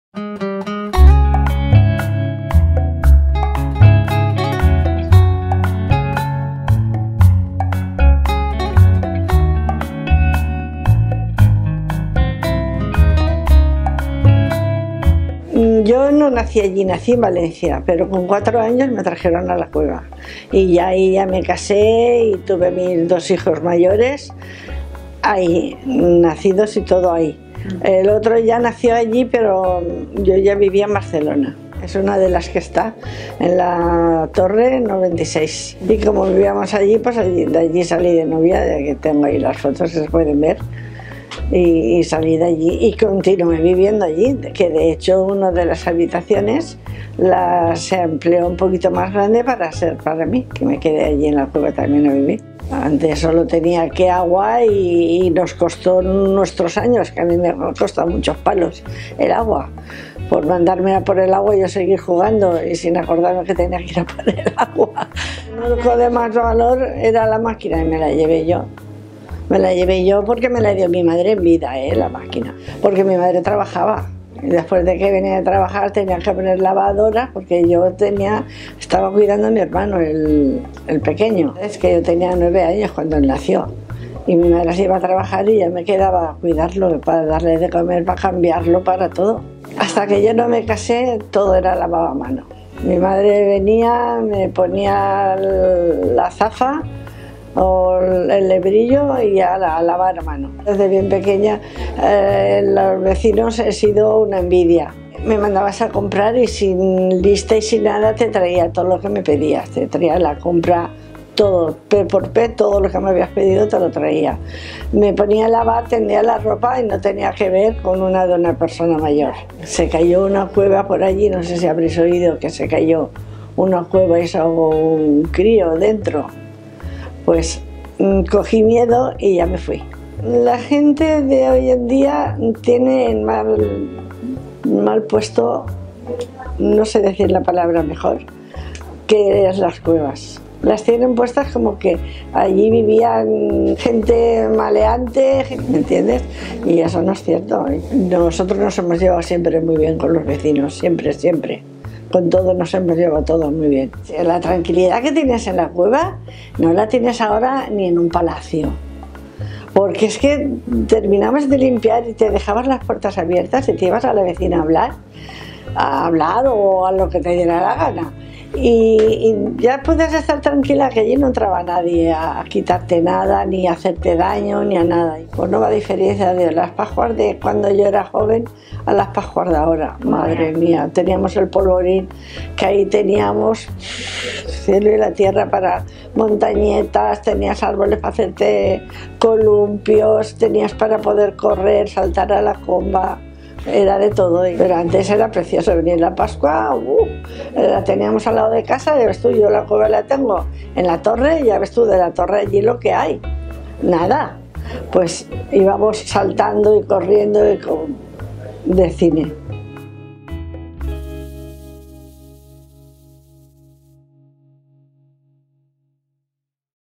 Mujeres Coveras Paterna - Fina Fernández Lucena.
“La tranquilidad de las cuevas no la tienes ni en un palacio” – Fina Fernández Lucena.

El calendario “Mujeres Coveras 2022-23” es una iniciativa de la concejalía de Inclusión Social y Políticas de Igualdad de Paterna.

Las cuevas vivienda son para Paterna un símbolo inconfundible, ligado a su identidad. Las experiencias de estas quince mujeres coveras nos cuentan la historia de nuestro municipio, sus raíces y sus tradiciones.

A través de este proyecto, el Ayuntamiento de Paterna rinde homenaje a todas y cada una de ellas, las que aparecen y las que no, poniendo en valor sus vivencias, sus aportaciones y el amor a su tierra.

Gracias a todas por escribir la historia de Paterna.